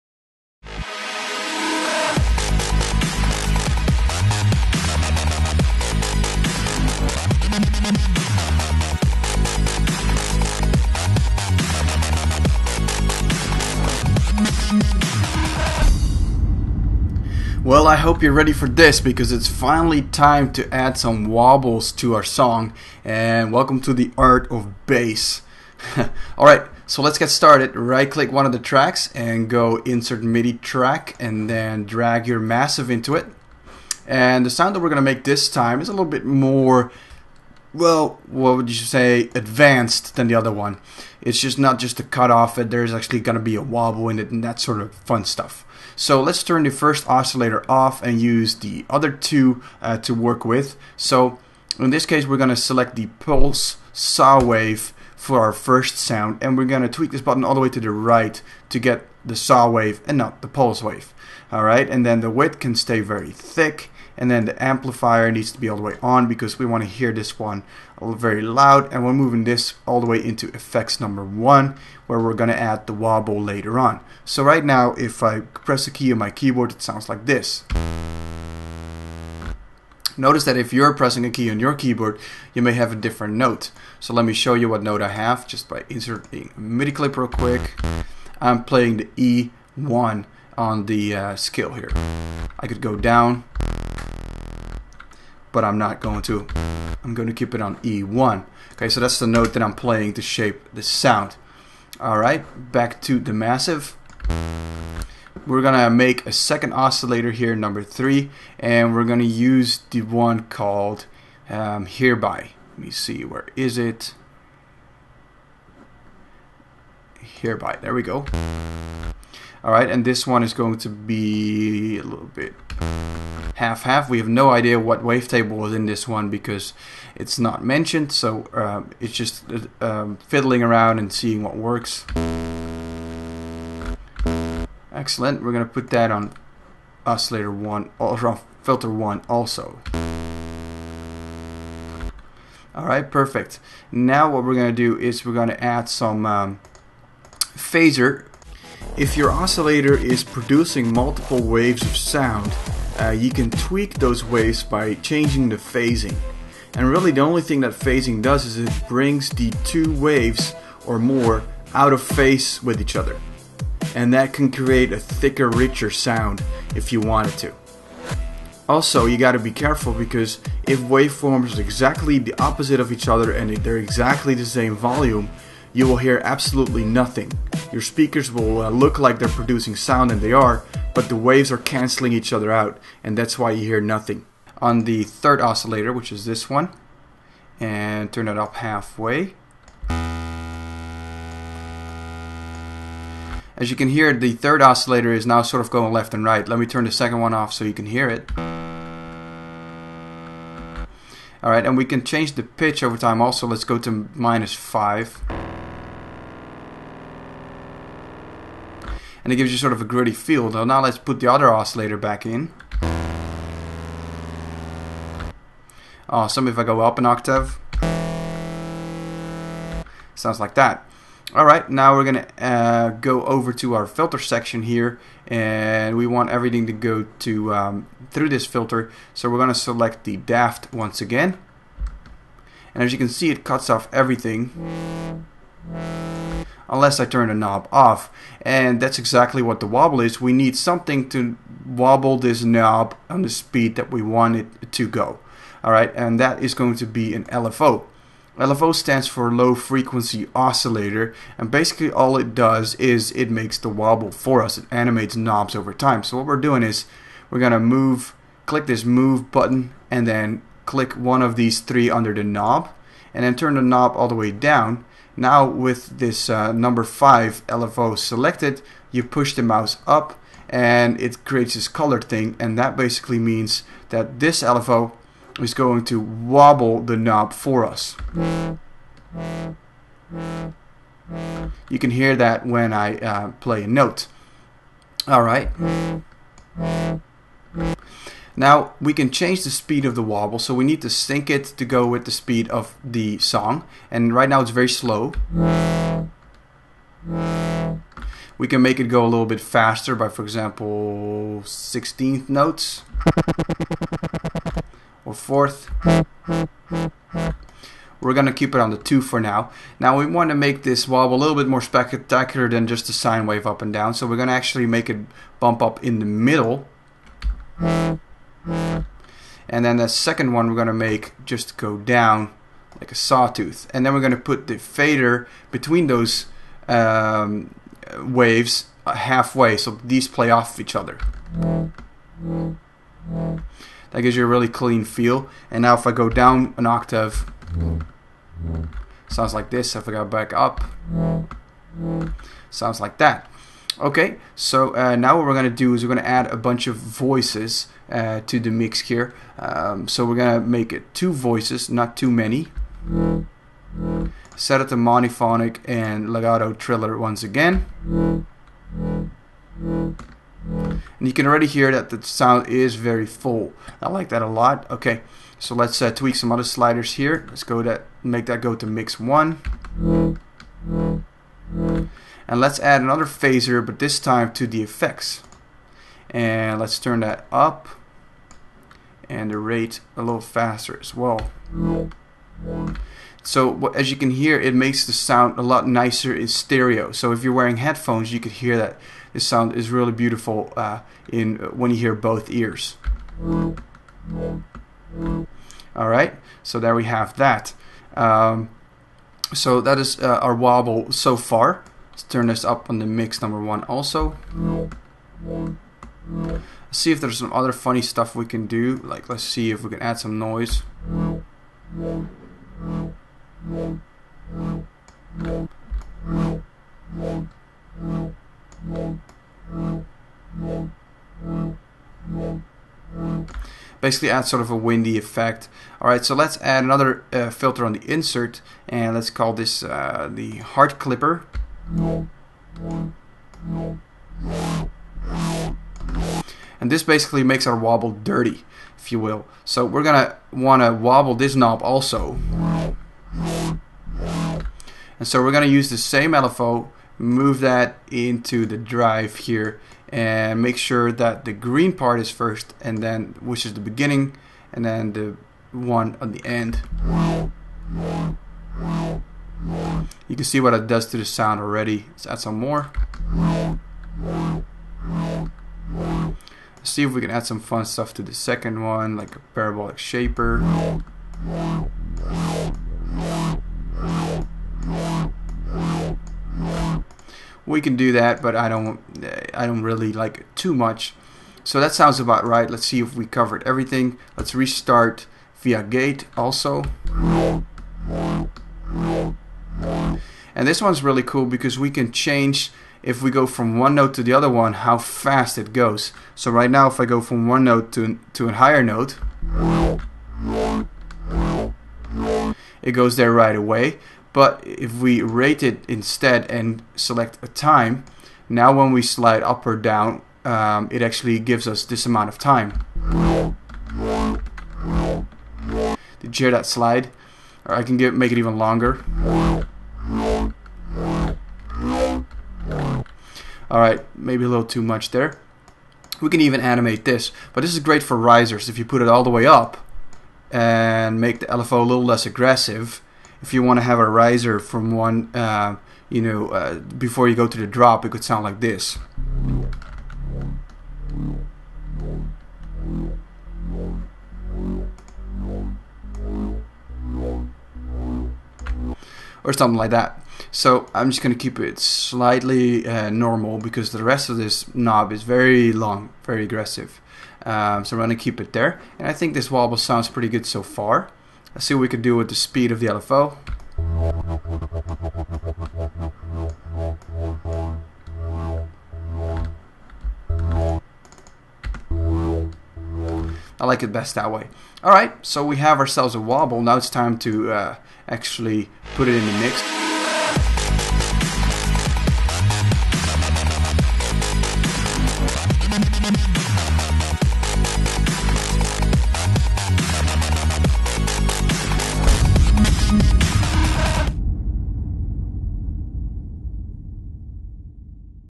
#11 How to make DUBSTEP like a pro (Ableton Live)
New mind-blowing tutorial by the TornadoTwins. Learn how to make electronic music from A-Z.
Dubstep is the fastest rising music genre in history. Learn how to make a killer track from beginning to end. Starting with drums, synths, wobbles and making the drop.